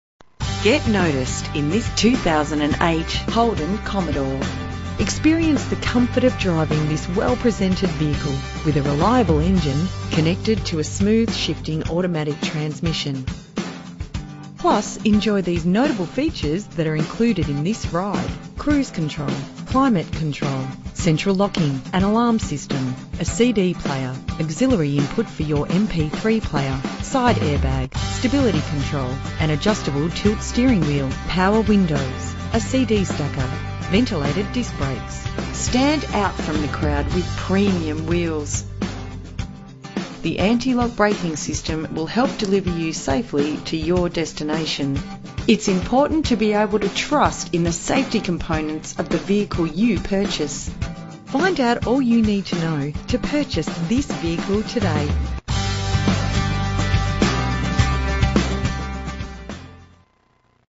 2008 HOLDEN Commodore SS - Burwood VIC
2008 HOLDEN Commodore SS 6 litre Sports Automatic Sedan SILVER Exterior.